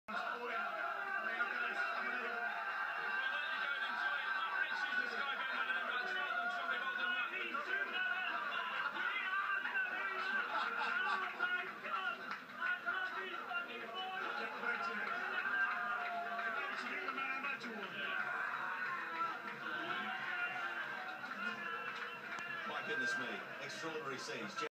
AFC Bournemouth vs Bolton 3 - 0 270415. HILARIOUS team & chairman celebrate Premiership.
Bournemouth vs Bolton - 270415 celebrating 3 - 0 near promotion to Premier League. Jeff Mostyn chairman goes crazy!!!  Hilarious!